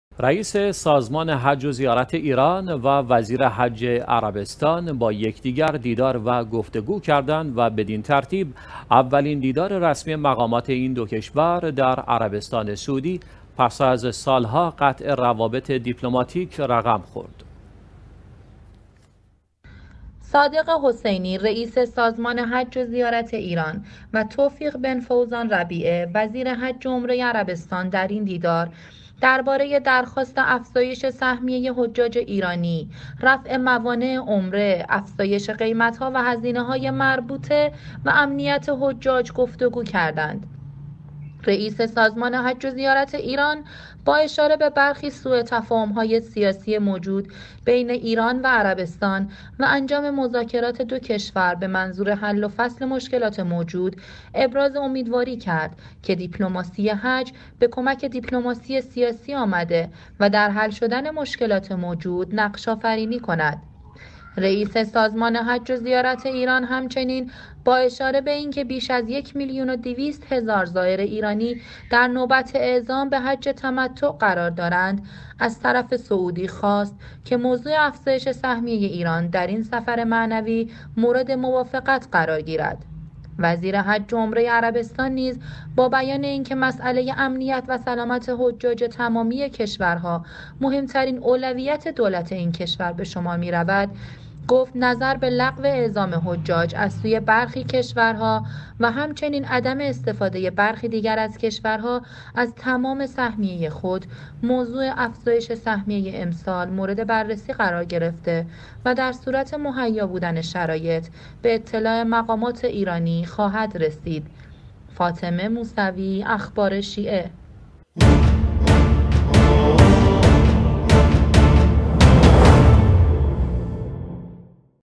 اولین دیدار رسمی مقام‌های ایران و عربستان پس از ۶ سال
رئیس سازمان حج و زیارت ایران و وزیر حج عربستان با یکدیگر دیدار و گفت وگو کردند و بدین ترتیب اولین دیدار رسمی مقامات این دو کشور در عربستان سعودی پس از سال‌ها قطع روابط دیپلماتیک رقم خورد.
صادق حسینی ، رئیس سازمان حج و زیارت ایران و توفیق بن فوزان ربیعه ، وزیر حج و عمره عربستان در این دیدار ، درباره درخواست افزایش سهمیه حجاج ایرانی ، رفع موانع عمره ، افزایش قیمت‌ها و هزینه‌های مربوطه و امنیت حجاج گفتگو کردند.
رئیس سازمان حج و زیارت ایران با اشاره به برخی سوء تفاهم‌های سیاسی موجود ایران و عربستان و انجام مذاکرات دو کشور به منظور حل و فصل مشکلات موجود ، ابراز امیدواری کرد که دیپلماسی حج به کمک دیپلماسی سیاسی آمده و در حل شدن مشکلات موجود نقش آفرینی کند.
رئیس سازمان حج و زیارت ایران همچنین با اشاره به اینکه بیش از یک میلیون و 200 هزار ایرانی در نوبت اعزام به حج تمتع قرار دارند ، از طرف سعودی خواست که موضوع افزایش سهمیه ایران در این سفر معنوی مورد موافقت قرار گیرد.
وزیر حج و عمره عربستان نیز با بیان اینکه مساله امنیت و سلامت حجاج تمامی کشورها مهم‌ترین اولویت دولت این کشور به شمار می‌رود ، گفت: نظر به لغو اعزام حجاج از سوی برخی کشورها و همچنین عدم استفاده برخی دیگر از کشورها از تمام سهمیه خود ، موضوع افزایش سهمیه امسال مورد بررسی قرار گرفته و در صورت مهیا بودن شرایط به اطلاع مقامات ایرانی خواهد رسید.